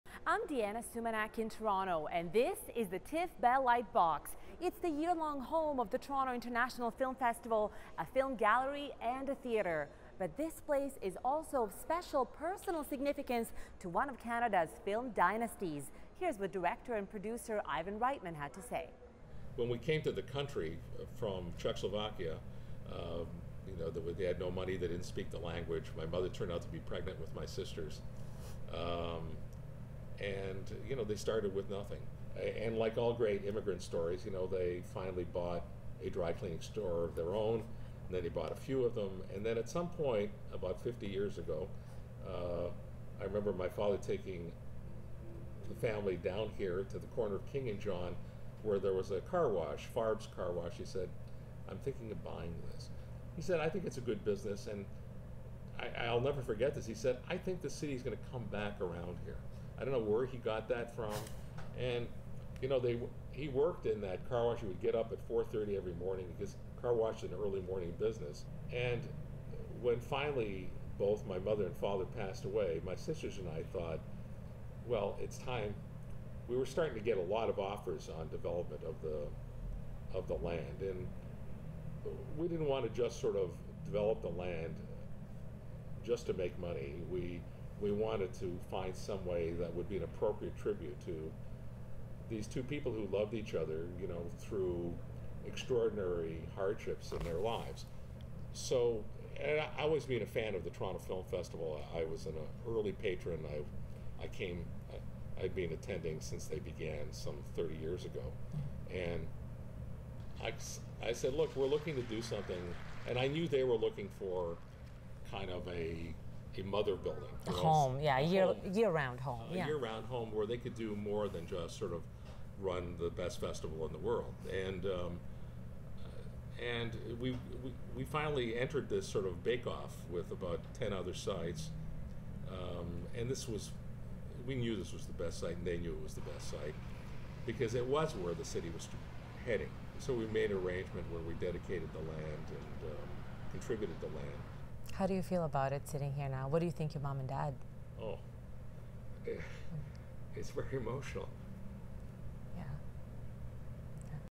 Ivan Reitman's personal connection to the TIFF Bell Lightbox
Description: Canadian film producer and director Ivan Reitman discusses his family's personal connection to the Toronto International Film Festival's headquarters.